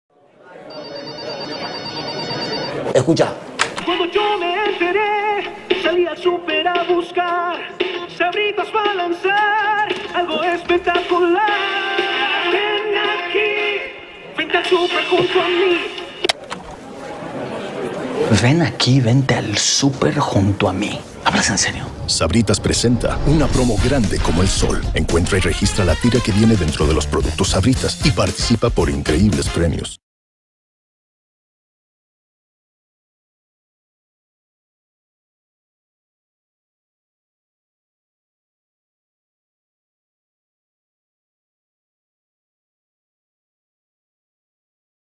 SABRITAS - Una Promo Grande Como El Sol (2021)
Comercial de Sabritas: Una Promo Grande Como El Sol.
Sabritas Luis Miguel La Serie.